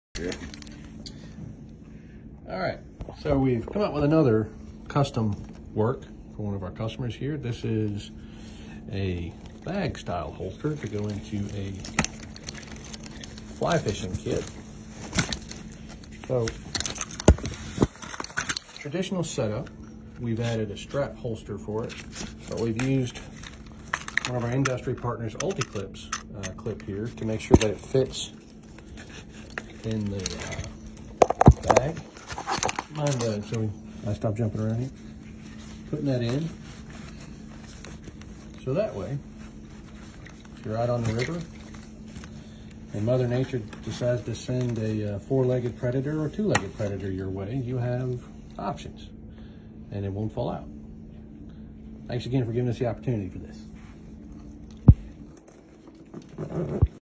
Fly Fishing Holster
Take a deeper look at a custom project we did for a customer's fly fishing bag! It's not often that we say no to a custom project, especially if you are local here in Georgia.